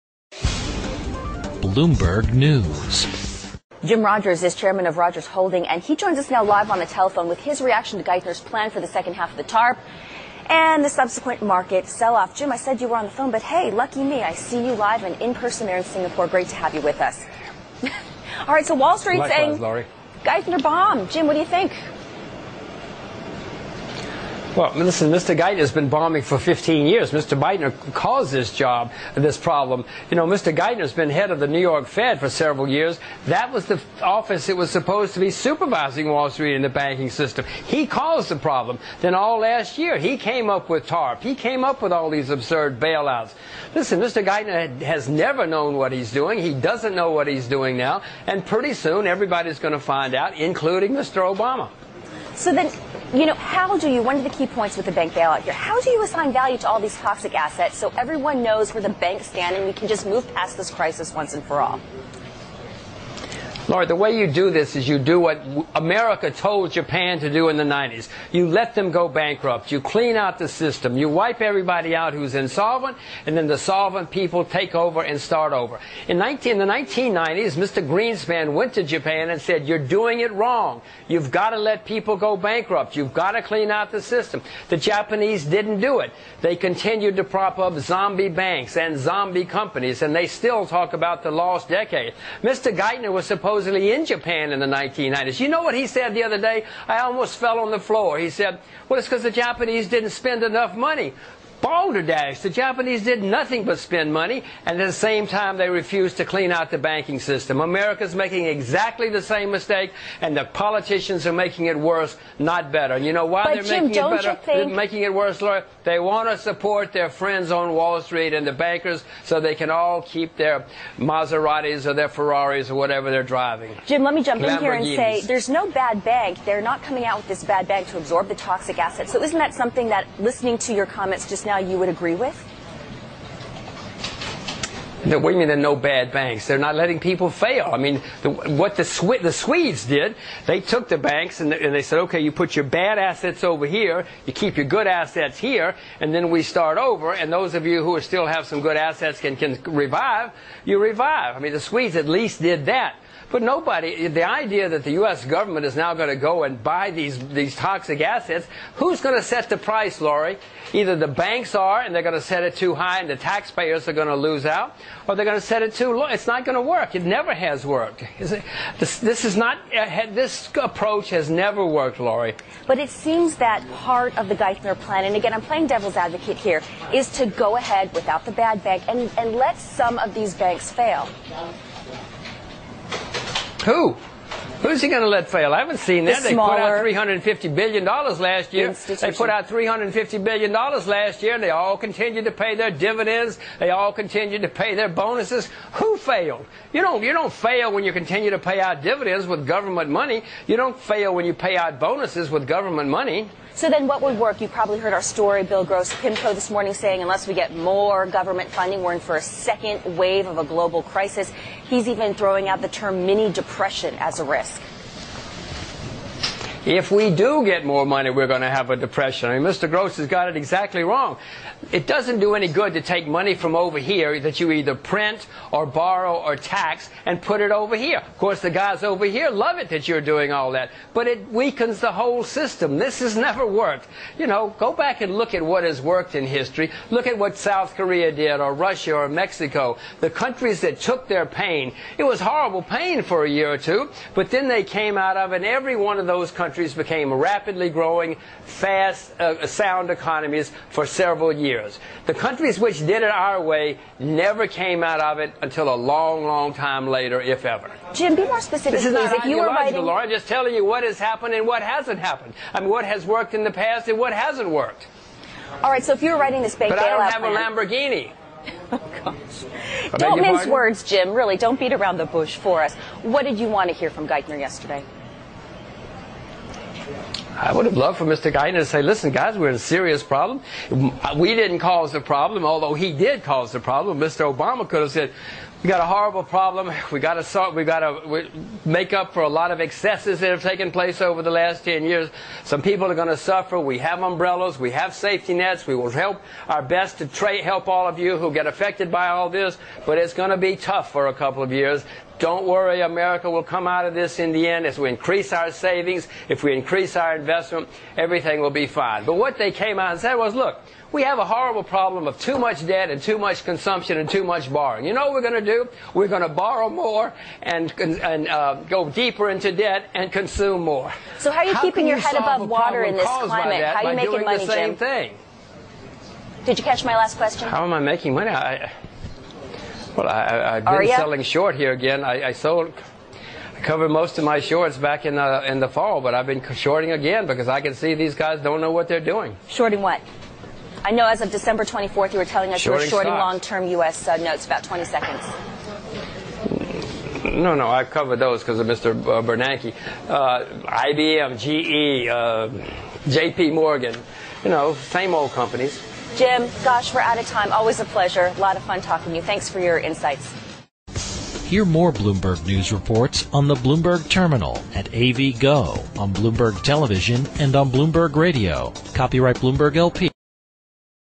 Jim Rogers Let the Banks Fail
02/11/09

"Mr Geithner's been bombing for 15 years. Mr Geithner caused this problem. Mr Geithner has been head of the New York Fed for several years - that was the office that was supposed to be supervising Wall Street and the banking system."

"He caused the problem."

"Then all last year he came up with TARP. He came up with all these absurd bailouts."

"The way you deal with this is you do what America told Japan to do in the 90's - you let them go bankrupt. You clean out the system. You wipe everybody out who's insolvent and then the solvent people take over and start over."

"You wanna know why they're making it worse? They want to support their friends on Wall Street and the bankers so they can all keep their Maseratis or their Ferraris or whatever they're driving."

"If doesn't do any good to take money from over here - that you either print, borrow, or tax - and put it over here. Of course the guys who get it, love that you're doing all that"

"Look at what's worked in history - South Korea, Russia, Mexico - they all took their pain. It was horrible pain for a year or two, but then they came out of it and every one of those countries became rapidly growing, fast, sound economies for several years."

"The countries that did it our way never came out of it until a long long time later if at ever"